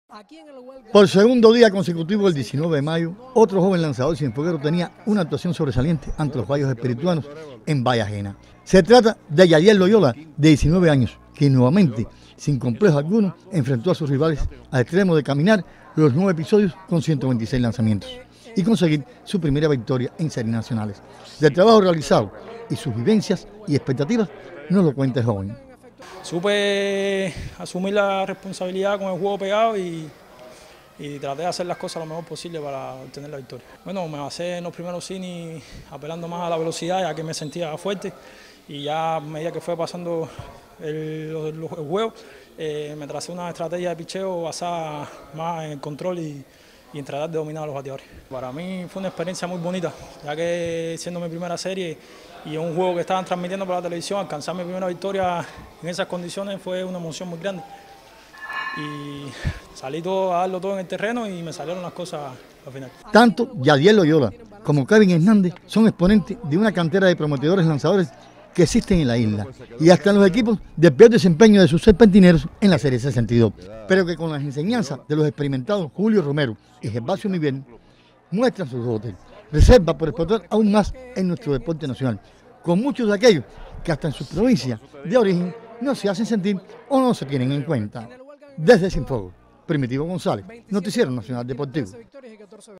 Despunta Yadiel Loyola Chávez como promesa de lanzadores de Elefantes de Cienfuegos
El joven lanzador Yadiel Loyola Chávez ganó su primer partido en tierras espirituanas en defensa de los colores de Cienfuegos en la actual Serie Nacional de Béisbol.